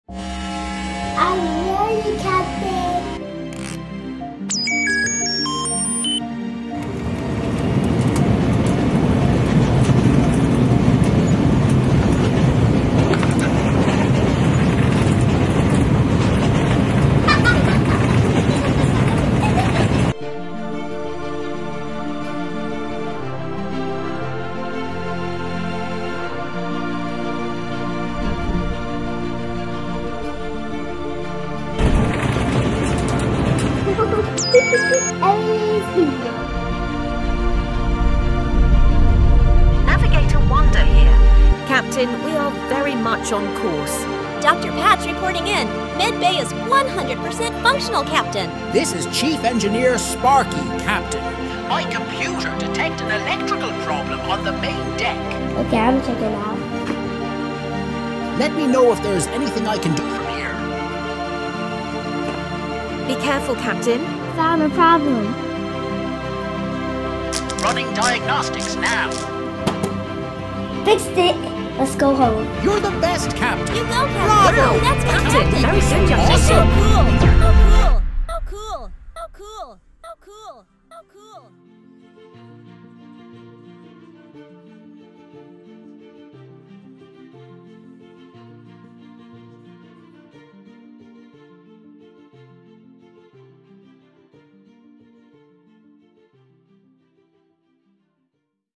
The Bedtime Comet
Stella Starbeam and Crew takes to the stars in this micro short film

Shot on Blackmagic Ursa Mini Pro
Rokinon cine Lens 24mm T1.5